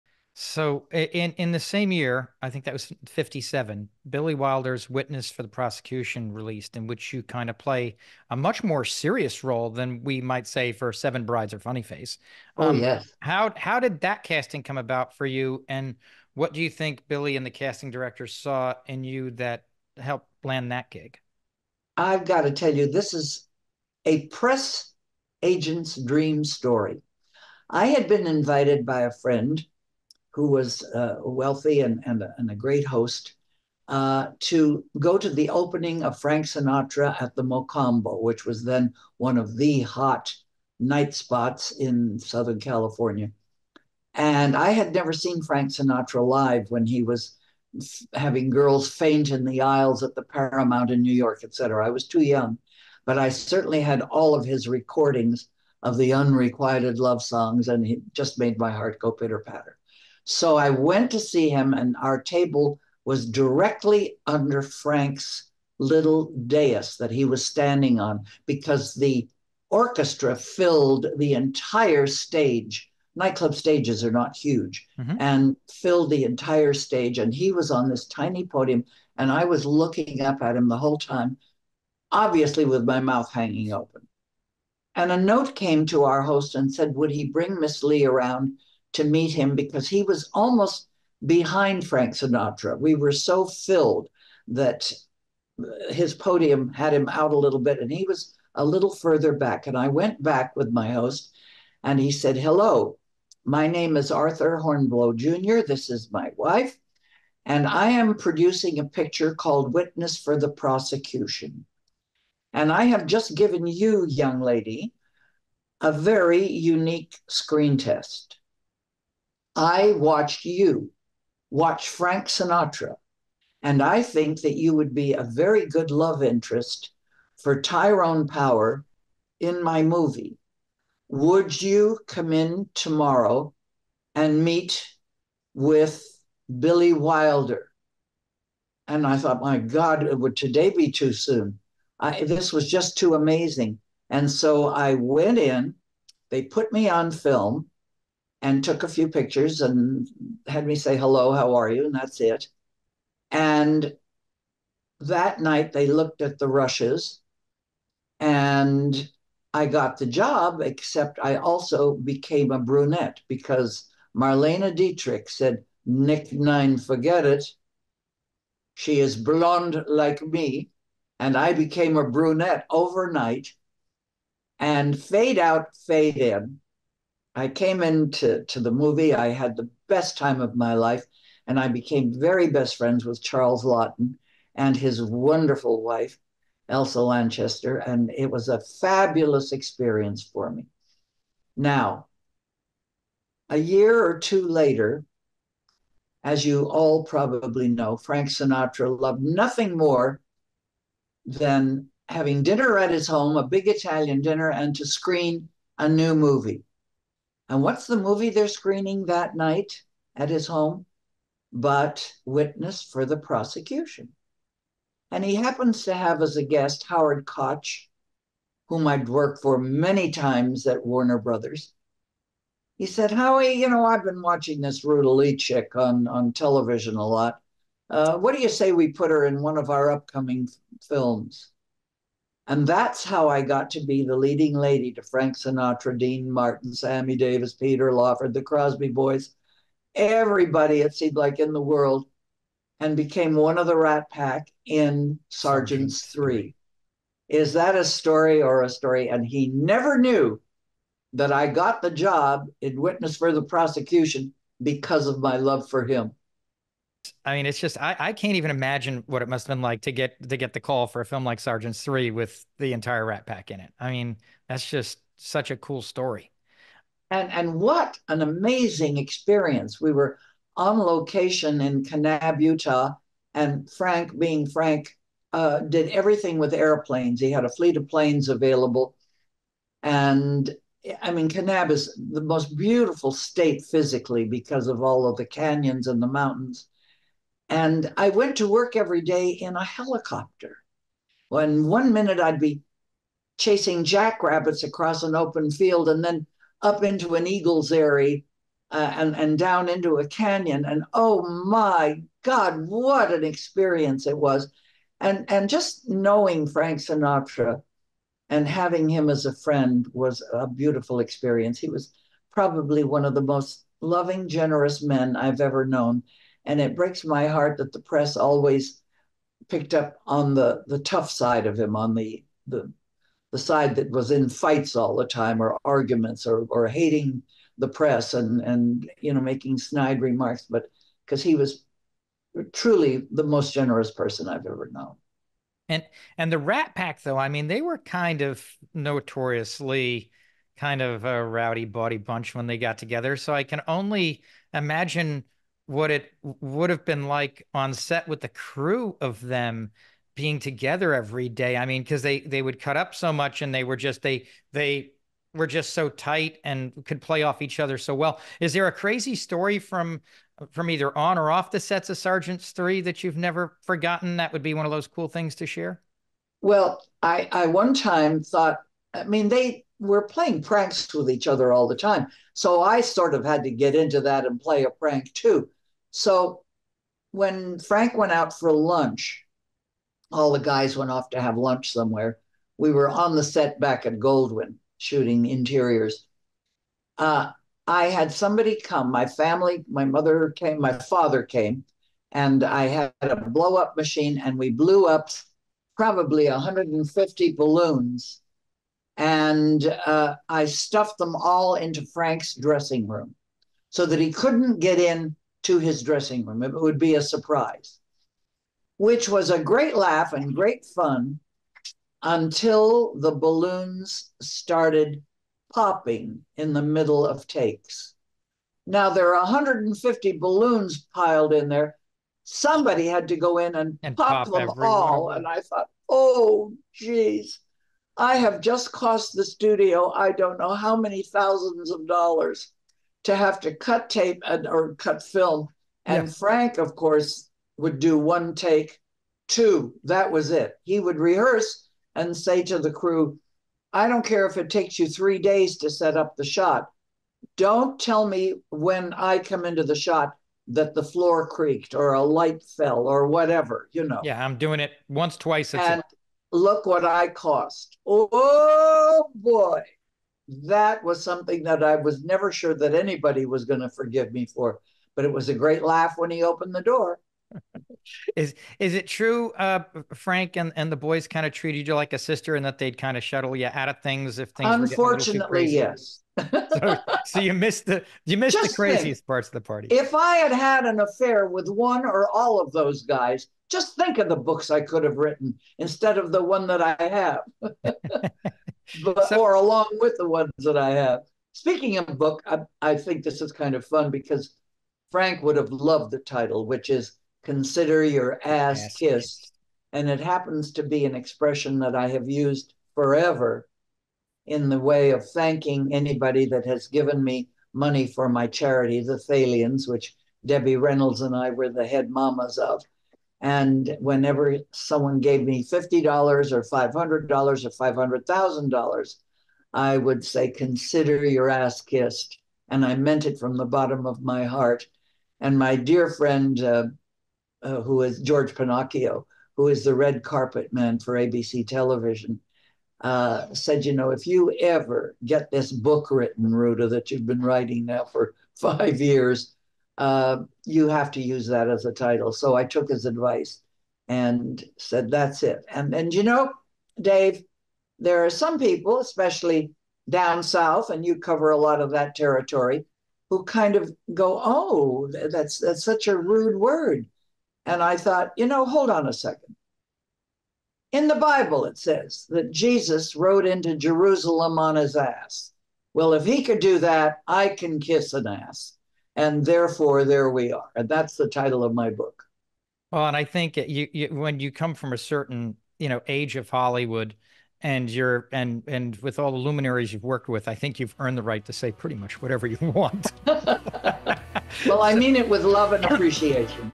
Ruta Lee on Frank Sinatra, Witness for the Prosecution, Sergeants 3
Ruta Lee on the incredible story of how she was "found" by a casting agent and landed "Witness for the Prosecution" at a Frank Sinatra performance, spending time with the Rat Pack on "Sergeants 3", Frank's pubic persona vs. his private persona, the pranks she pulled on set that almost got her fired, and so much more.